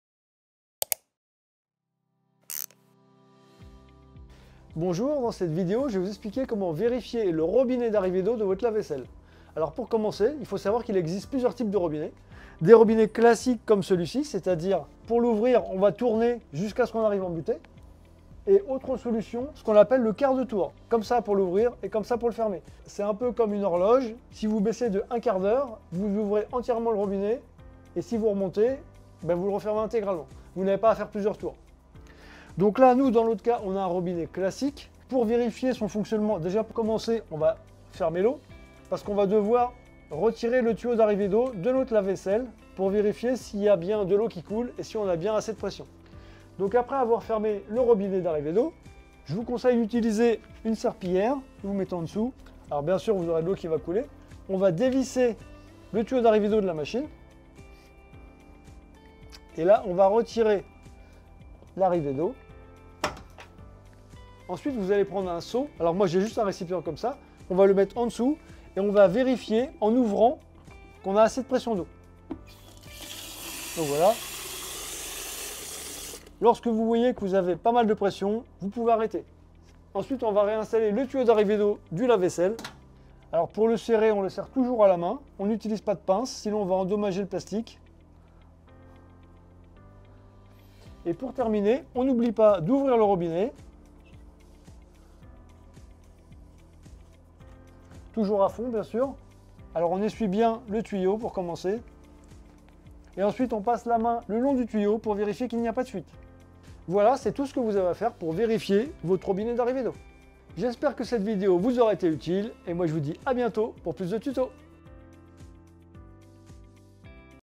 Lave-vaisselle et panne d'eau : Comment vérifier le robinet d'arrivée d'eau !
Dans cette vidéo nous allons vous montrer comment vérifier le robinet d'arrivée d'eau de votre lave-vaisselle.
Le robinet peut être de type classique ou à quart de tour en fonction de votre installation.
Dans le cas ou vous avez un défaut d'arrivée d'eau sur votre appareil, un code erreur va s'afficher sur le bandeau de commande de votre lave vaisselle.
Vous devrez commencer par fermer le robinet d'arrivée d'eau en tournant dans le sens des aiguilles d'une montre et dévisser le tuyau de votre lave-vaisselle.
Mettez un récipient sous le robinet et ouvrez pour vérifier s'il y à suffisamment de pression.
Si le test est bon revissez le tuyau et ouvrez le robinet d'arrivée d'eau en tournant dans le sens inverse des aiguilles d'une montre, passez la main le long du tuyau pour vérifier qu'il n'y a pas de fuite.